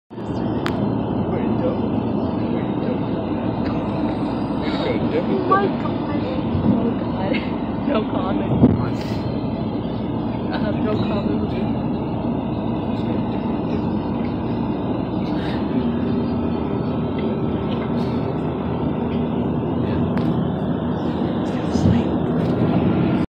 PS man is Going to Universal Orlando to have me a great time part(3)
Posing for the selfie with the group and when my friend. Welcome to Orlando with the group at Universal Studios Orlando resort these are the videos of the trip part